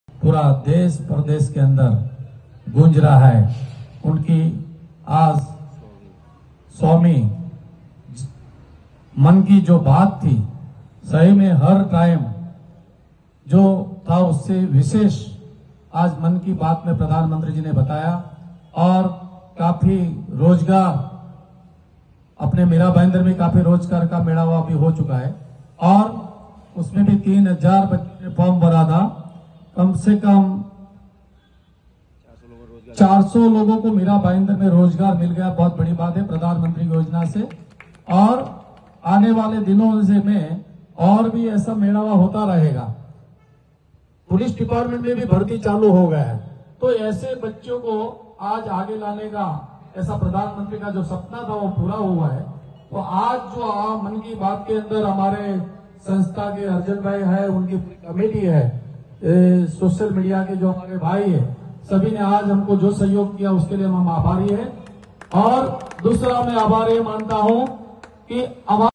प्रधान मंत्री जी की मन की बात के सोवे एपिसोड का मीरारोड में भव्य कार्यक्रम
Video from hello gujarat Tv